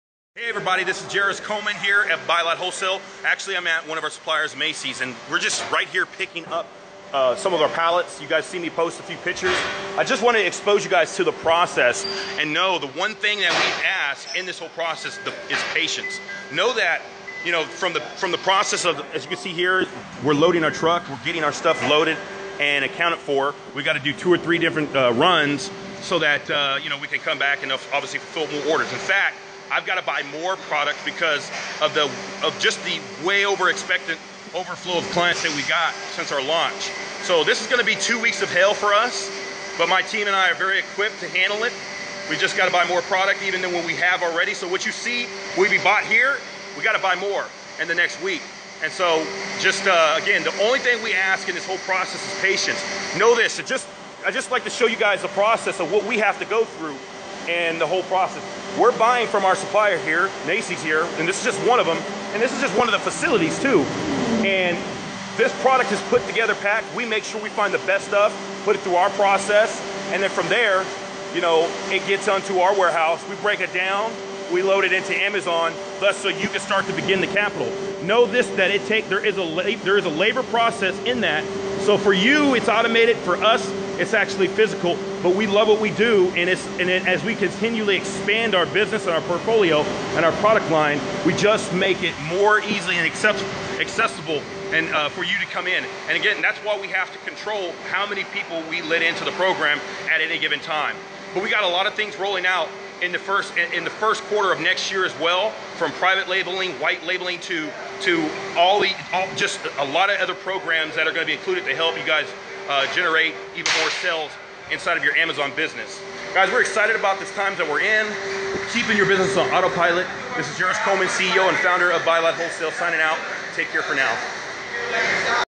Automated Amazon Business (behind the scenes)
A sneak peek at one of the warehouses we purchase products from, Macy's. Start your own business with our done for your Amazon selling system.